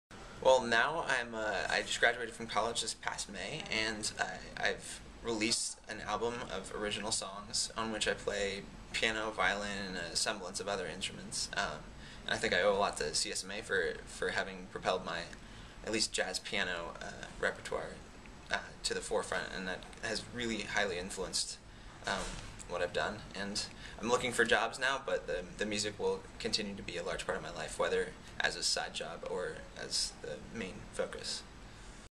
CSMA Alum, Scott Jesperson, (Part 3)
Alum, Scott Jesperson, talks about studying violin and jazz piano at CSMA.